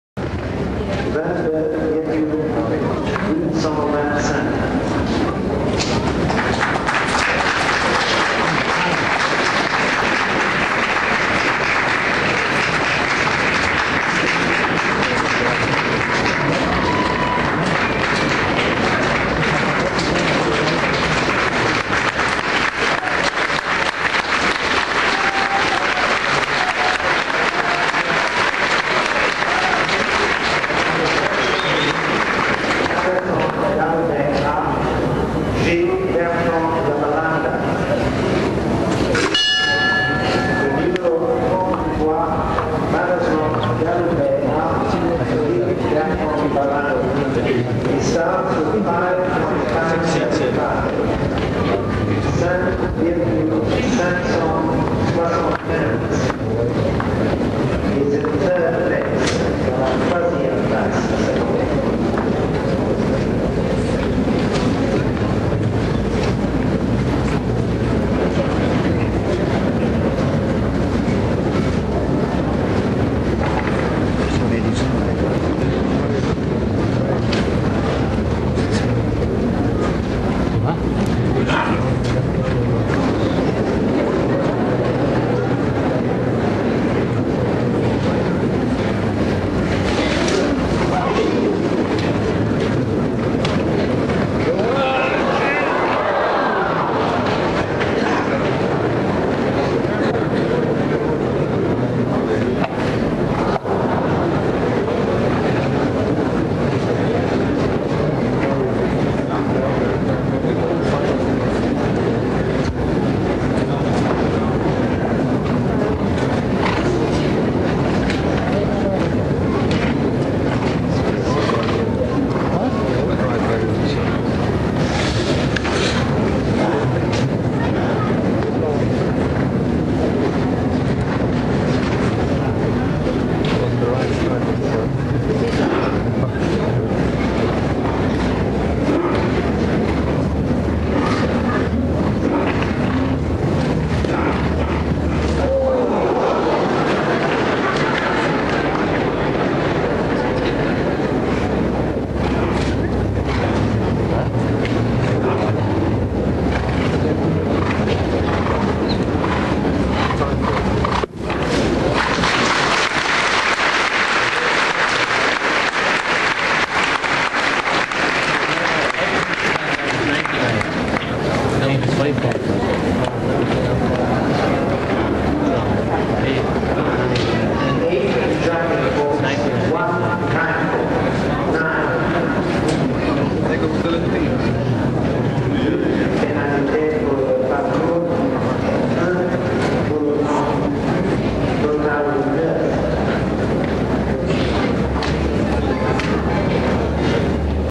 Galoubet A & Gilles Bertrand de Balanda World Cup 82 Ireland 1st round
Haras de Montfort archive ( Brazil )  .
To the young generation who only heard about his best son . The fantastic Galoubet A with an incredible rider during a magic moment . Enjoy !!
Pour la nouvelle generation qui a seulement ecouté sur son meilleur fils . Le fantastique Galoubet A avec un super cavalier pendant un moment magique !! Profitez .
Haras de Montfort - Brèsil .